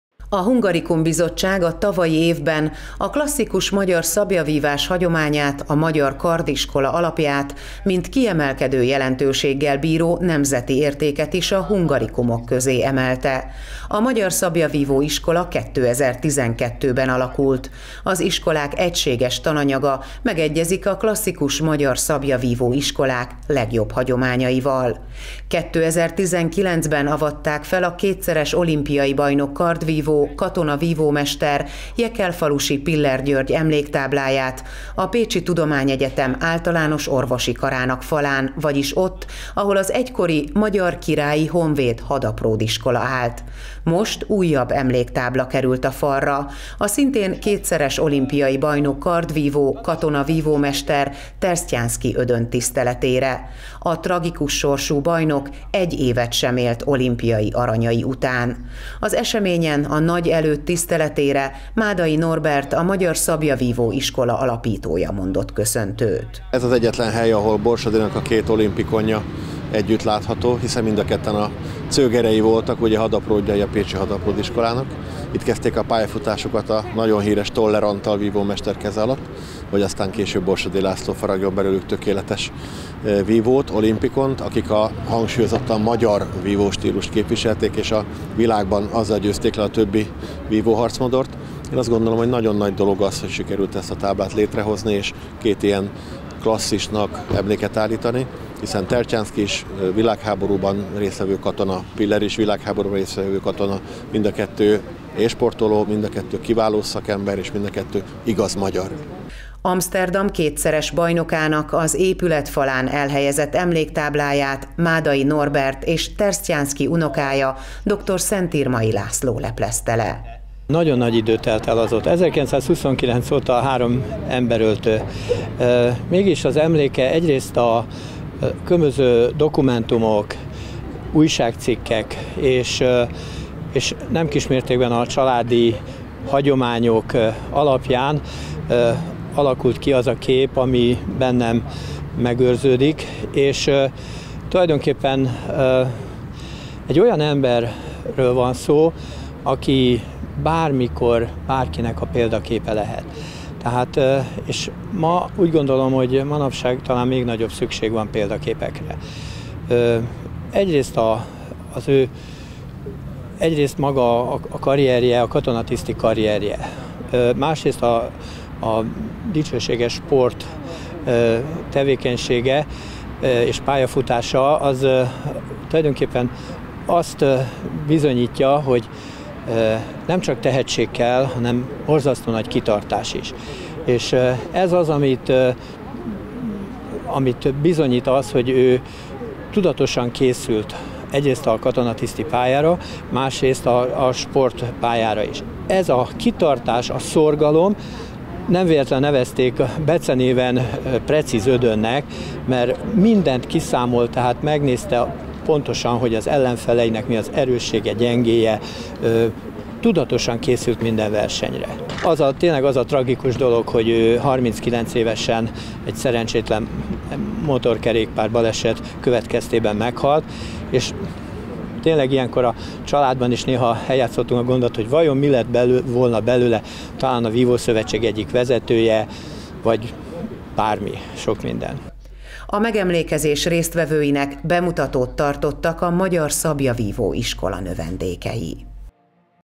Emléktáblát kapott Tersztyánszky Ödön - a kardvívó egy évet sem élt olimpiai aranyai után
Emléktáblát kapott Tersztyánszky Ödön - a tragikus sorsú kardvívó egy évet sem élt olimpiai aranyai után

A kétszeres olimpiai bajnok kardvívó, katona vívómester, Tersztyánszky Ödön, abban a Magyar Hadapródiskolában végzett 1917-ben, amely a Pécsi Tudományegyetem Általános Orvosi Karának helyén állt. Az épület falán elhelyezett emléktábláját a Magyar Szablyavívó Iskola alapítója, Máday Norbert és Tersztyánszky unokája, Dr. Szentirmay László leplezte le. A tragikus sorsú kardvívó egy évet sem élt olimpiai aranyai után.